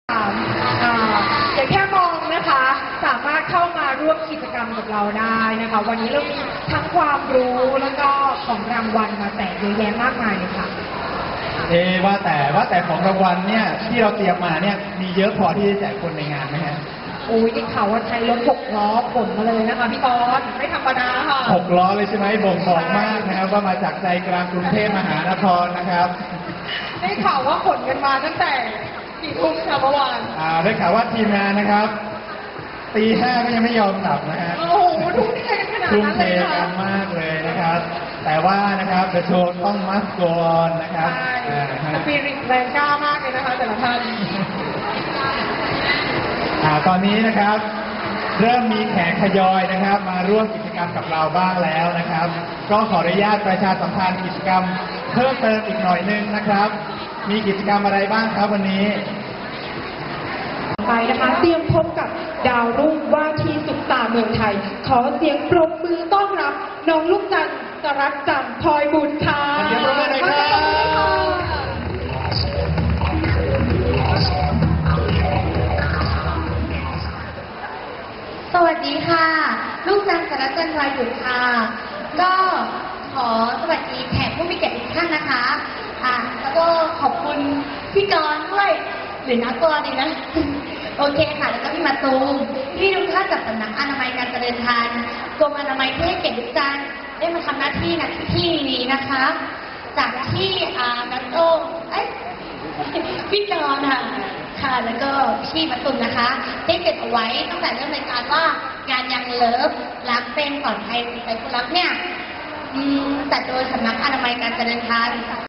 ลูกจันทร์ ศรัจจันทร์ พลอยบุศย์ @ งาน Young Love รักเป็น ปลอดภัย ใส่ใจคนรัก [12.2.15]
ลูกจันทร์ ศรัจจันทร์ พลอยบุศย์ @ งาน Young Love รักเป็น ปลอดภัย ใส่ใจคนรัก ณ Siam Discovery [12.2.15]